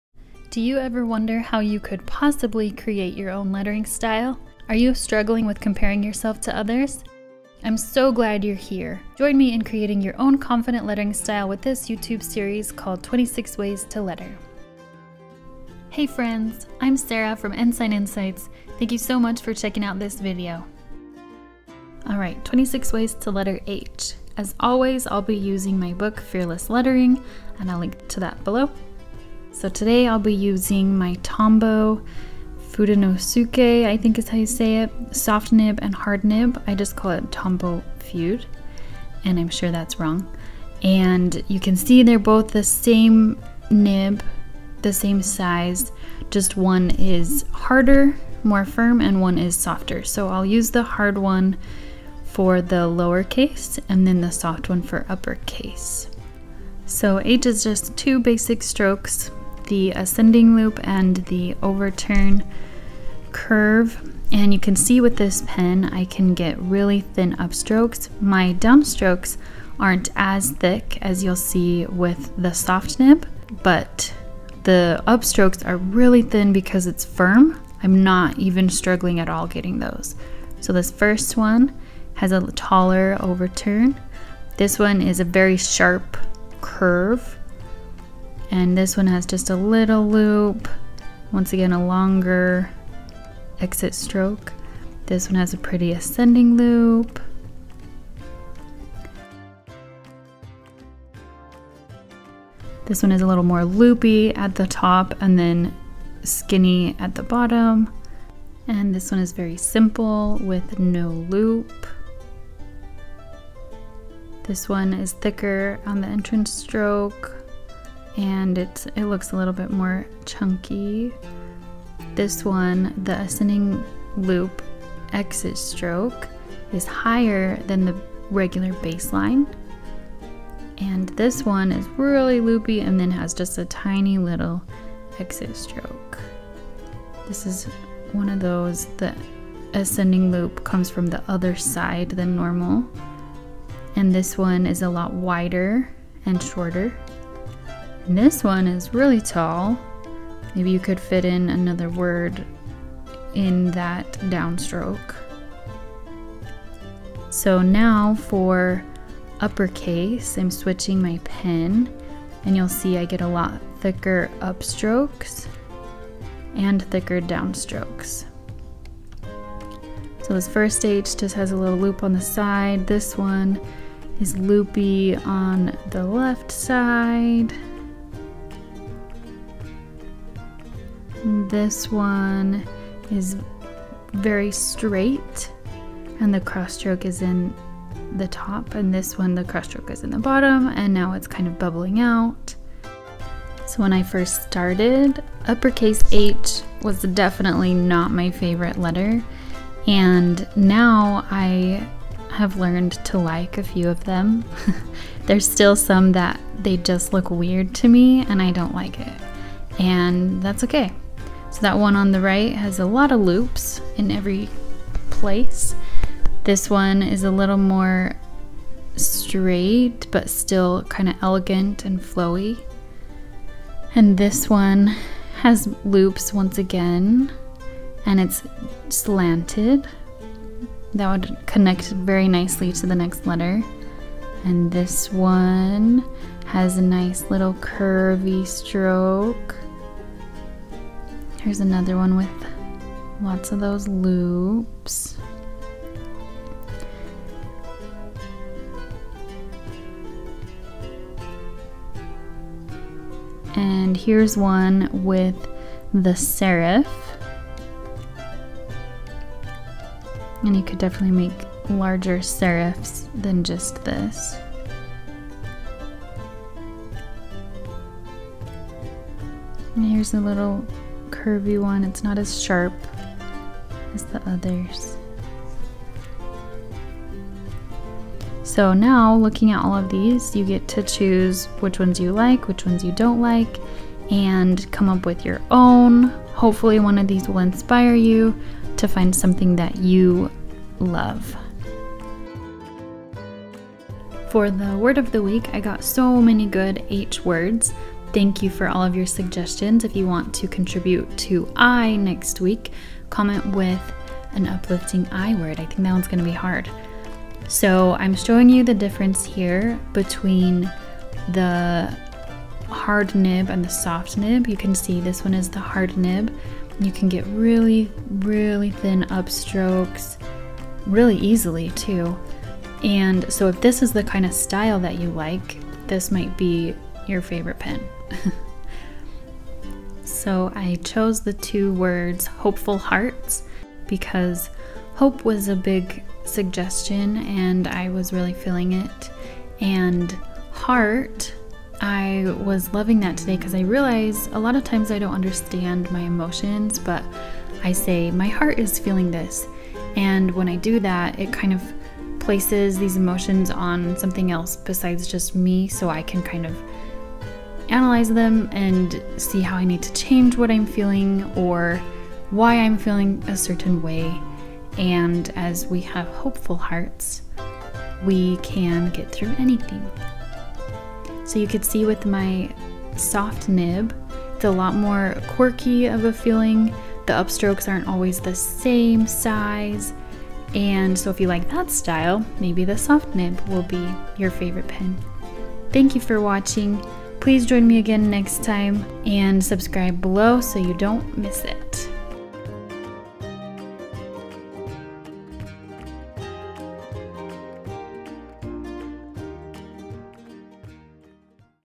How to find your lettering style: 26 Ways To Letter H in uppercase and lowercase
Comment with an uplifting word starting with I and I will choose one to letter in my video next week!

Thanks for joining the 26 Ways To Letter YouTube series! The goal is to help you learn how to find your own hand lettering style. This week is how to letter H in uppercase and lowercase.

If you try it and post to Instagram, use the hashtag fearlesslettering or fearlessfridayletters.